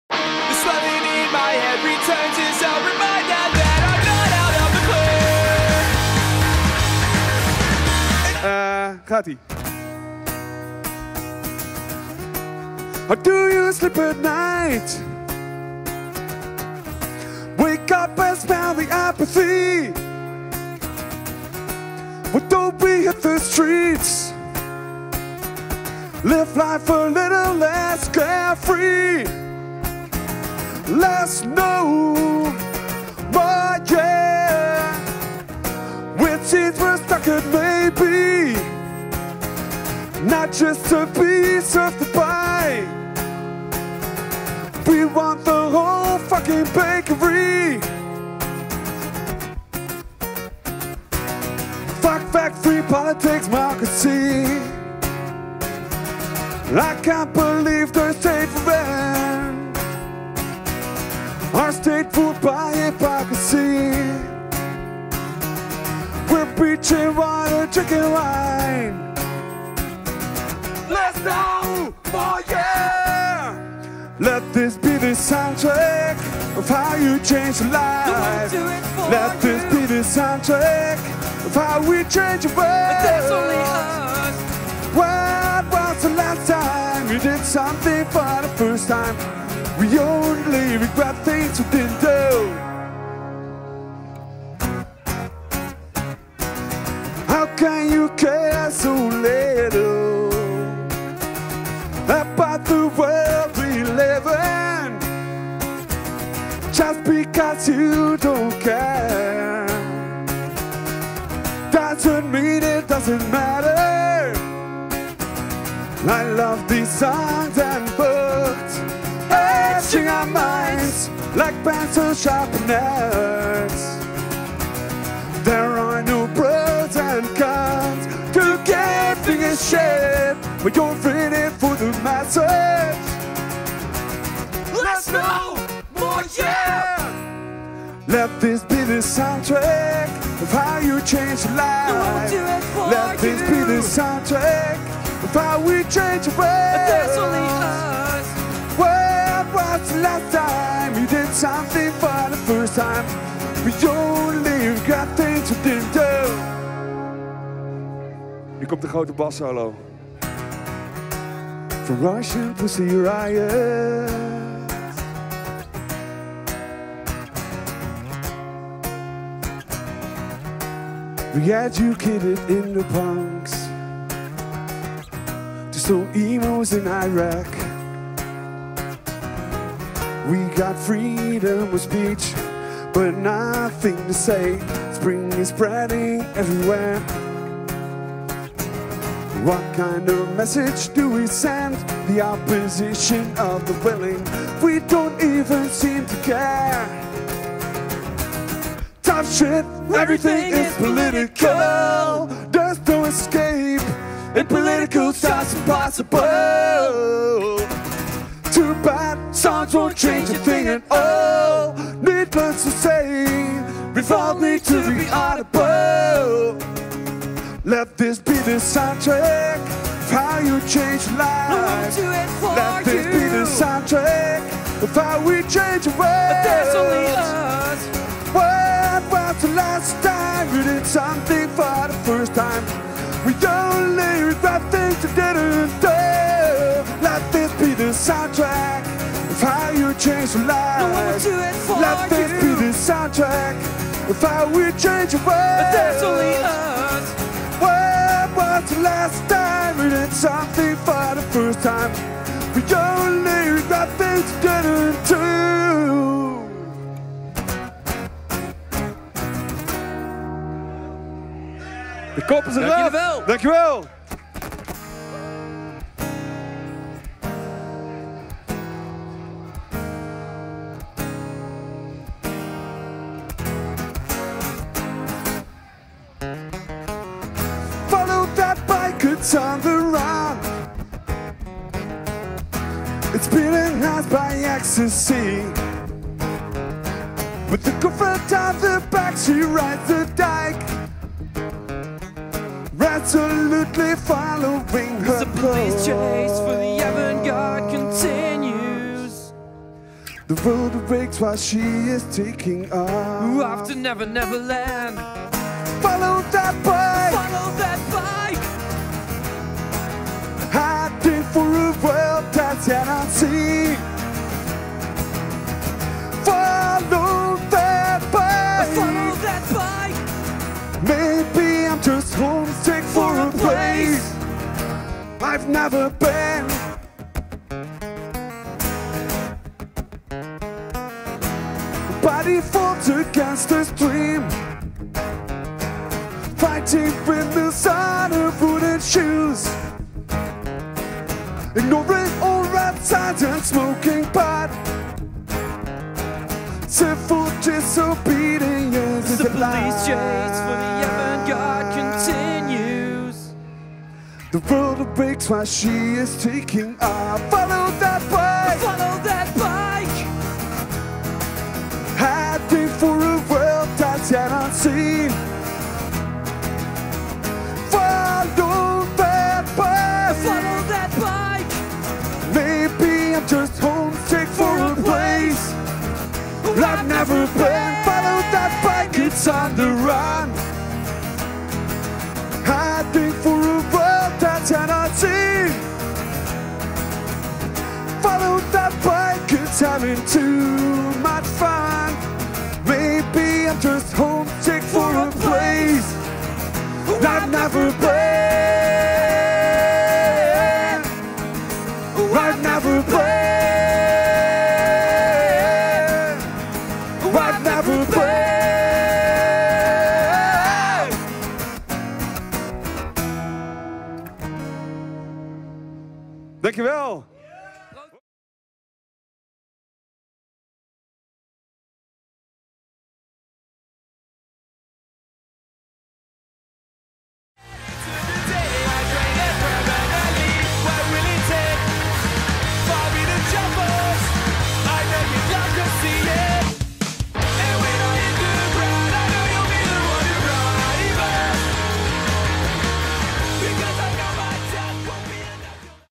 Mom's Living Room & Punktastic @ Jera On Air 2016 - Antillectual
Just Like Your Mom & Punktastic proudly present Acoustic sessions in Mom’s Living room @ Jera On Air 2016

During 2016’s Jera On Air Festival (NL) Just Like Your Mom, American Socks and Punktastic have teamed up to host acoustic sessions by a whole lotta bands who are featured on the Jera On Air bill.

For the sessions we dressed up Mom’s catering stall to serve as Mom’s Living Room. The bands play a few tunes, we film it for future generations and Mom gives them shelter, vegan food and an oat drink.

Video/Editing

Production by Kamikaze-PR
Bart Wismans & Alfred van Luttikhuizen